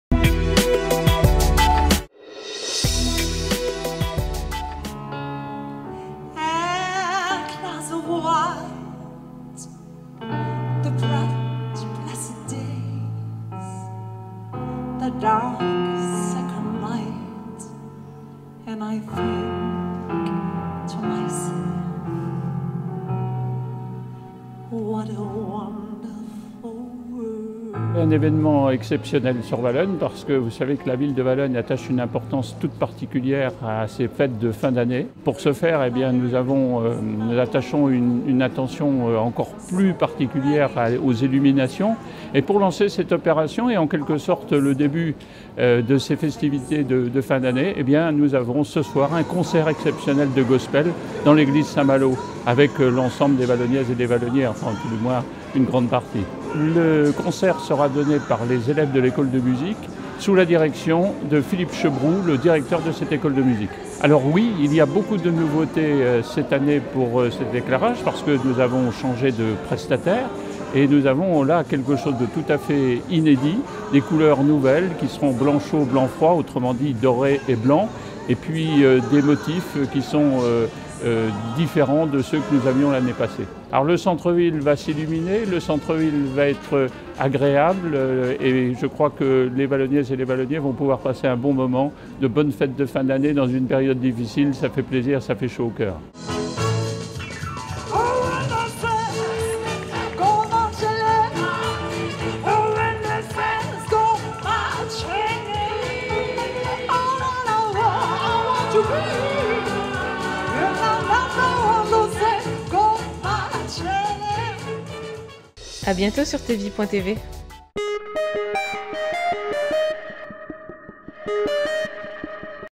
VALOGNES (50) mise en lumière et concert
Lancement des festivités de fin d'année à Valognes, dans le Cotentin. Images des illuminations et du concert Gospel.